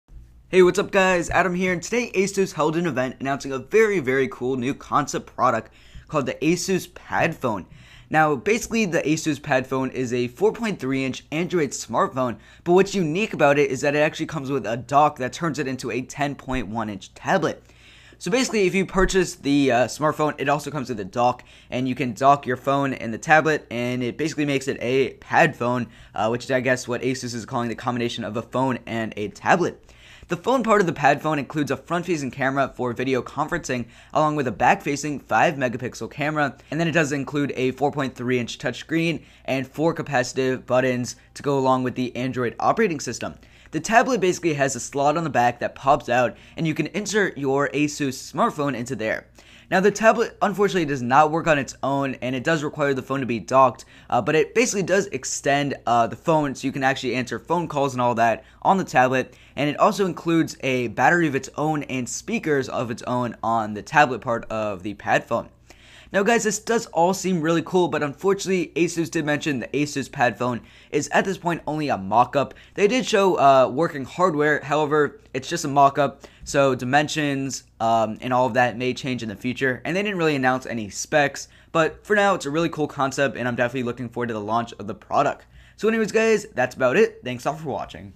Asus Padfone Phone/Tablet Overview
Asus Padfone Phone/Tablet First Look

In this video I go over the brand new Asus Padfone, the new product Asus announced this morning that combines both a smartphone and a tablet. The concept shows a 4.3 inch smartphone that can be docked into a 10.1 inch tablet and be used through there.